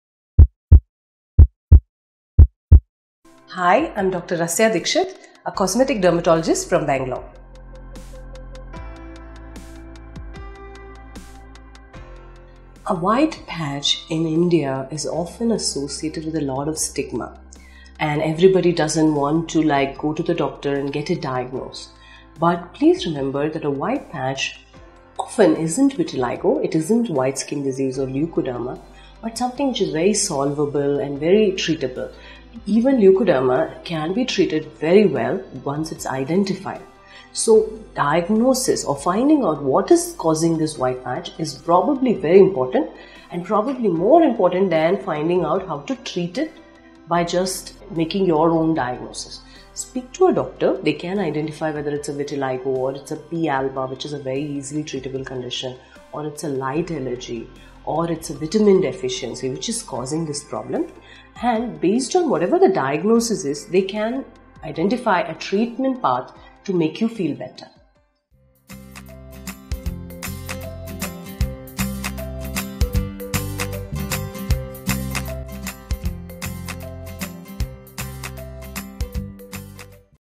How can one manage white patch on upper corner of lip? - Dr. Rasya Dixit
A white patch in India is often associated with a lot of stigma and everybody doesn’t want to go to doctor and get it diagnosed. Remember that the white patch is not often vitiligo, it is not white skin disease or leukoderma, but something which is very solvable and treatable. Even leukoderma can be treated very well once it is identified. So the diagnosis or finding out what is causing this white patch is probably very important and more important that finding how to treat it by just making your own diagnosis. Speak to a doctor. They can identify whether it is a vitiligo or a P alba which is an easily treatable condition or a light allergy or a vitamin deficiency which is causing the problem and based on whatever the diagnosis is they can identify a treatment path to make you feel better.